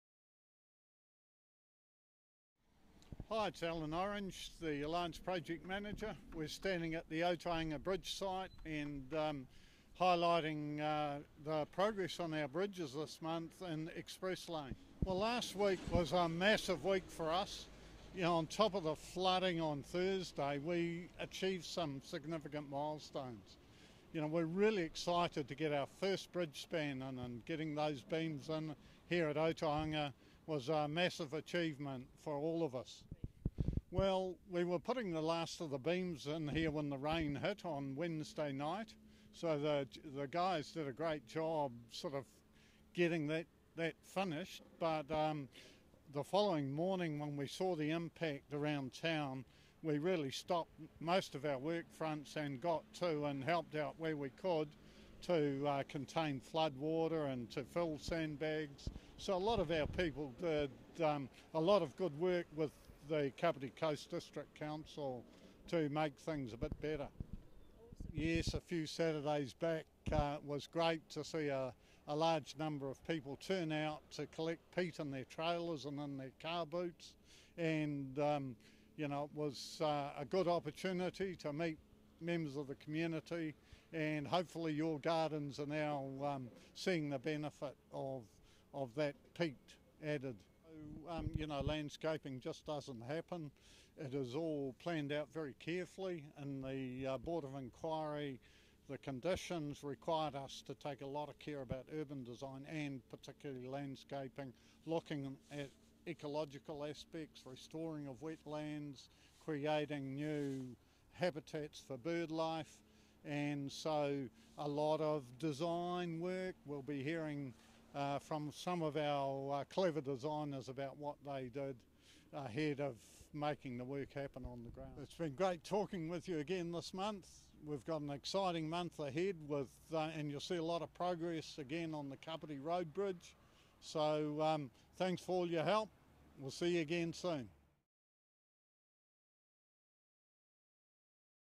Alan Orange - M2PP Project Manager
Reporting from Otaihanga Road for Express Lane, May 2015.